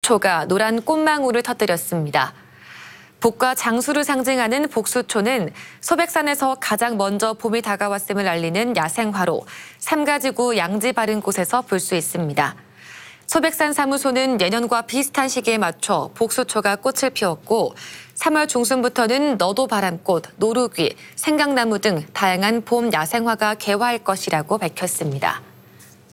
[아침]소백산에서 '봄의 전령' 복수초 개화/ 안동MBC
2024/02/20 12:00:00    작성자 : 김건엽

소백산에서 '봄의 전령' 복수초가
노란 꽃망울을 터뜨렸습니다.

복과 장수를 상징하는 복수초는
소백산에서 가장 먼저 봄이 다가왔음을
알리는 야생화로 삼가지구 양지바른 곳에서
볼 수 있습니다.

소백산사무소는 예년과 비슷한 시기에 맞춰
'복수초'가 꽃을 피웠고 3월 중순부터는
너도바람꽃, 노루귀, 생강나무 등
다양한 봄 야생화가 개화할 것이라고
밝혔습니다..